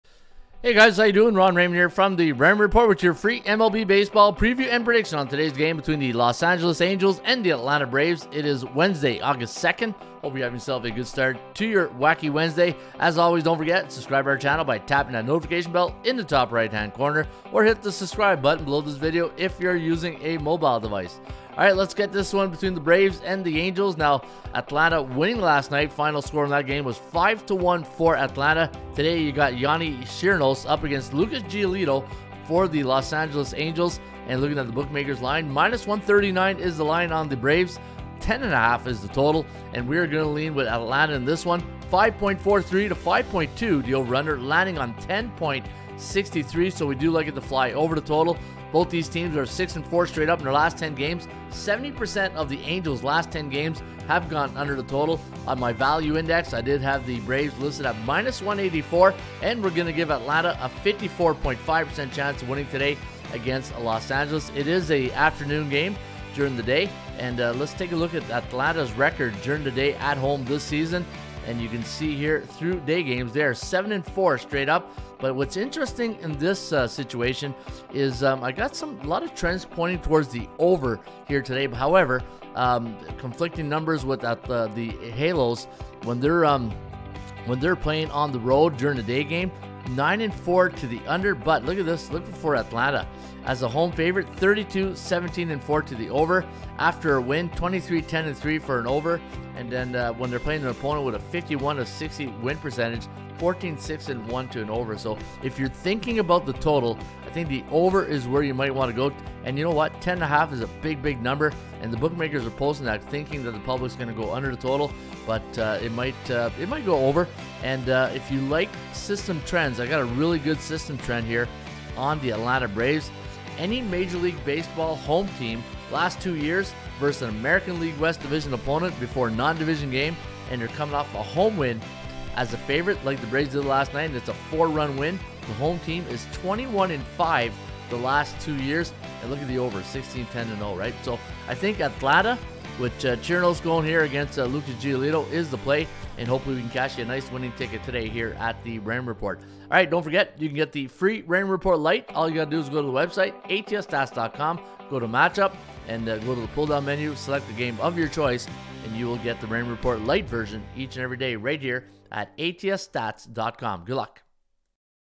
Los Angeles Angels vs Atlanta Braves Prediction 8/2/23 - Free MLB Picks
🔥⚾ Los Angeles Angels vs. Atlanta Braves Prediction 📅 August 2, 2023 ⚾🔥

Get ready for another thrilling interleague matchup as the Los Angeles Angels take on the Atlanta Braves in what promises to be an exciting MLB game! 🏟️💥

Looking for expert insights and FREE MLB picks? 📈 Look no further! We've got you covered with a detailed analysis and top-notch predictions for this highly-anticipated contest! 📊💪

Will the Angels' dynamic offense continue to shine against the Braves' pitching staff, or will Atlanta's talented hitters lead them to victory against Los Angeles' pitchers? Get ready for a clash filled with excitement and intense competition! 🤩

Remember to bet responsibly and enjoy this fantastic baseball showdown between two competitive teams! 🔥📺